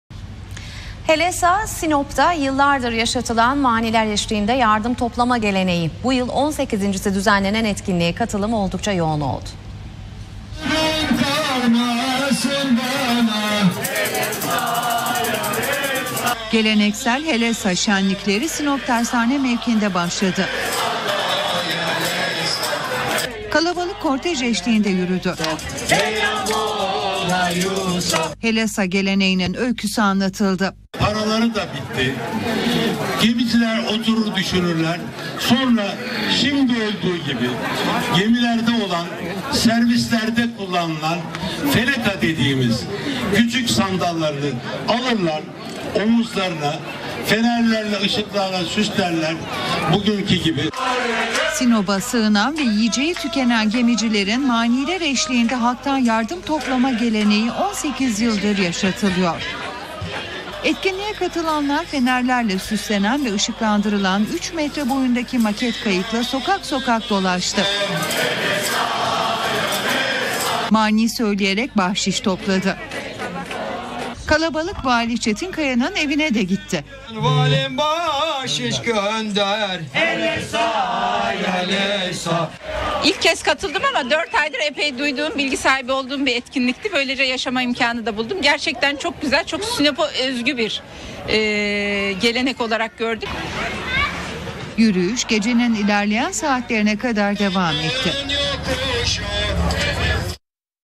SİNOP'TA HELESA GELENEĞİNİN 18.Sİ YAPILDI
Sinop’a özgü bir gelenek olan ve her yıl ramazan ayında düzenlenen helesa geleneğinde Sinoplular sokaklara dökülüyor.